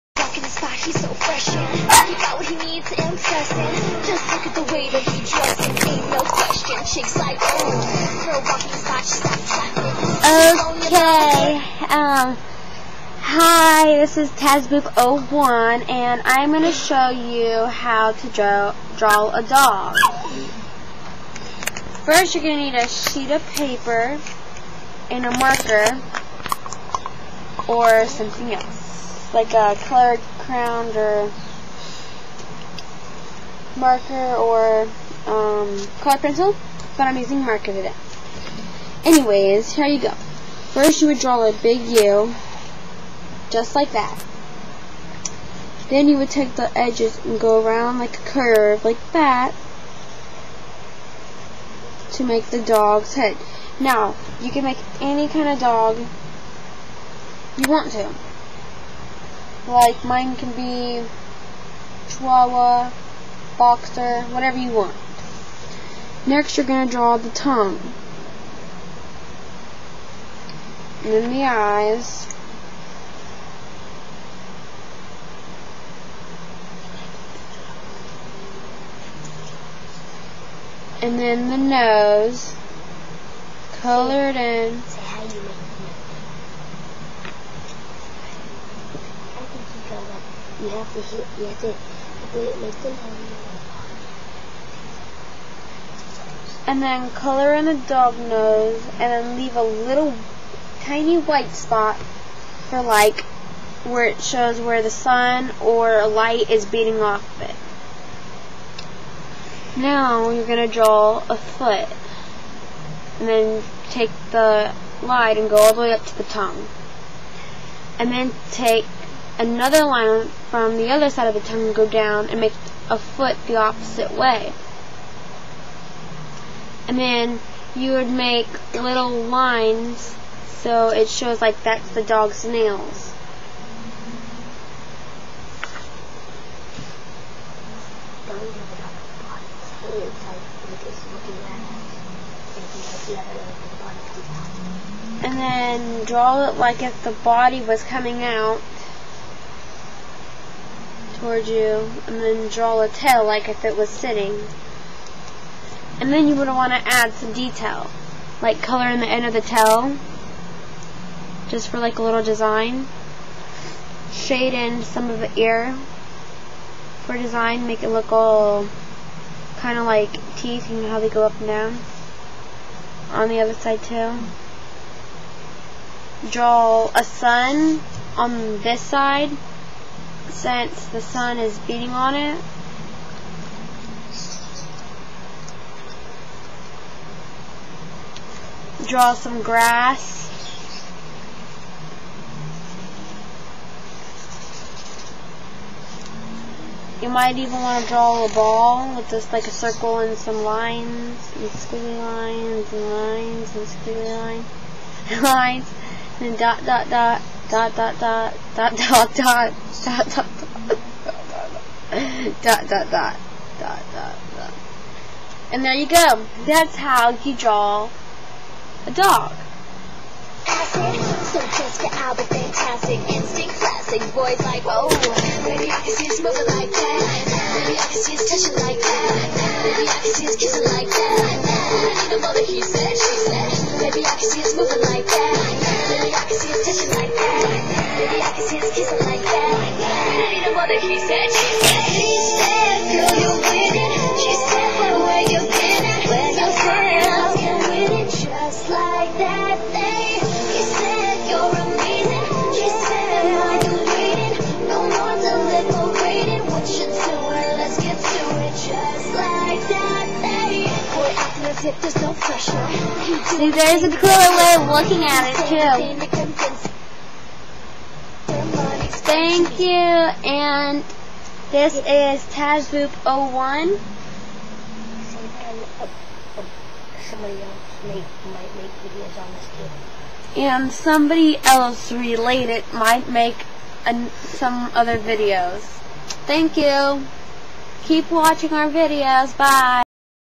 easy dog drawings
if you like to draw this is your thing to watch. watch the video and draw as you watch fav my video